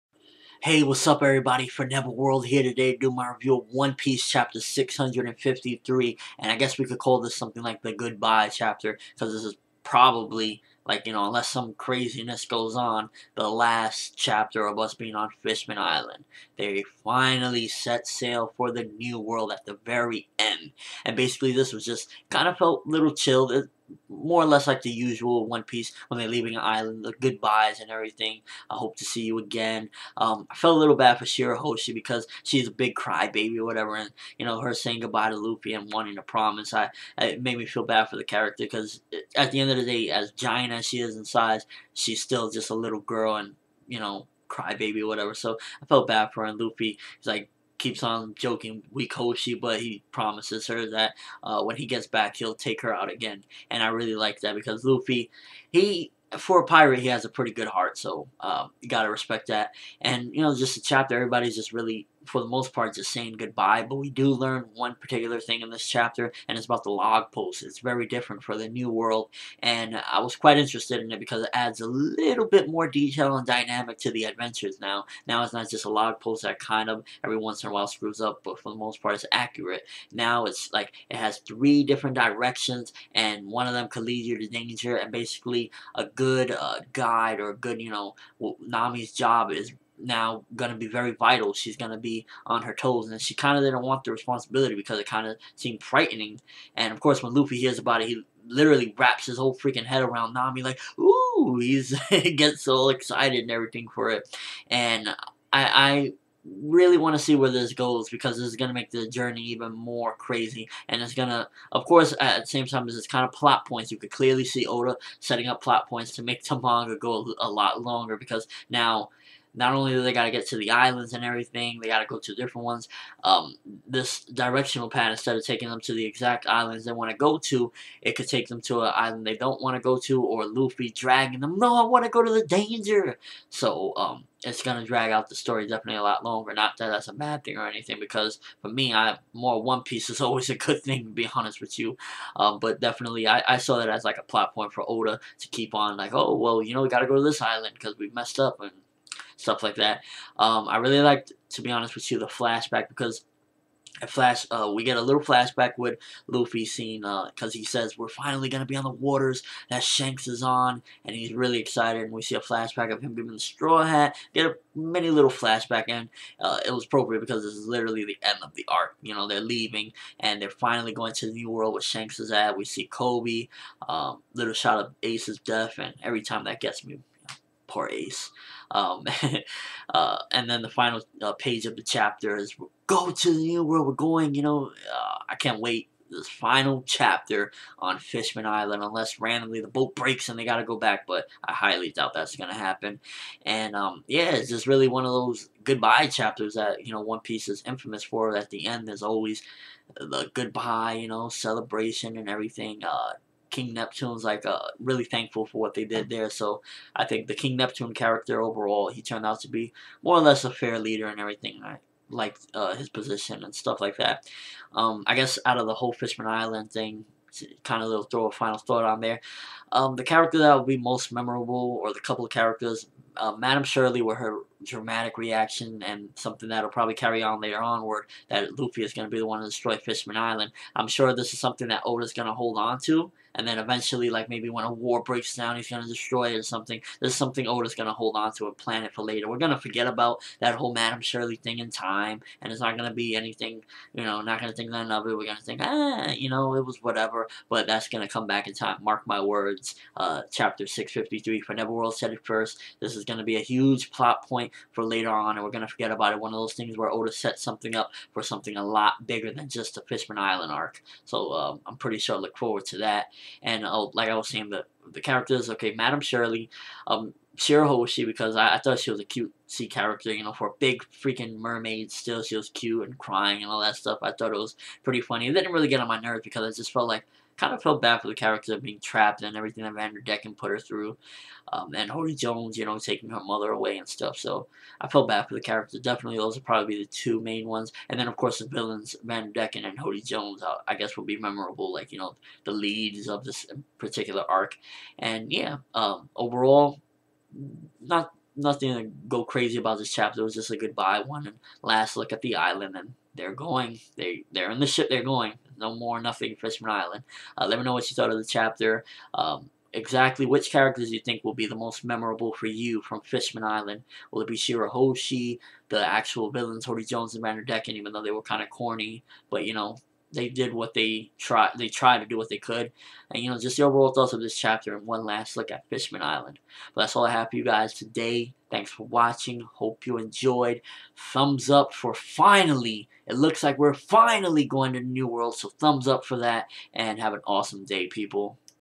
One Piece 653 Manga Chapter Review- Good Bye Fishman Island, To The New World ワンピース
What Did You Think of Chapter 653?